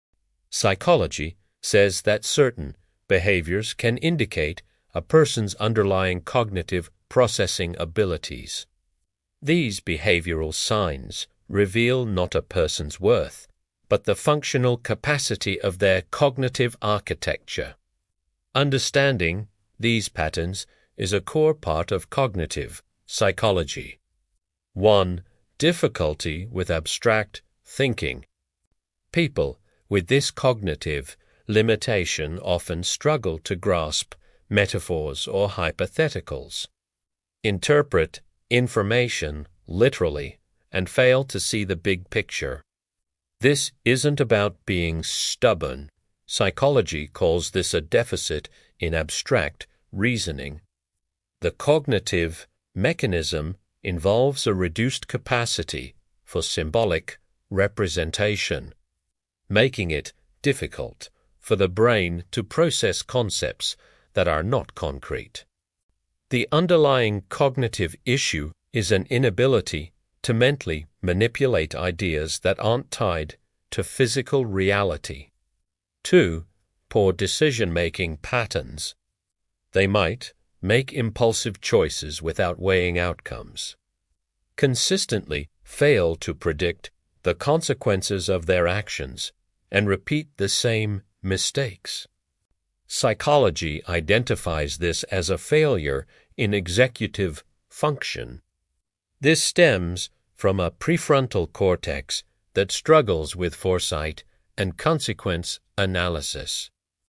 Signs of Extremely Low IQ
What are the behavioral signs of extremely low IQ? Psychology and cognitive science explain the specific patterns that indicate lower cognitive functioning.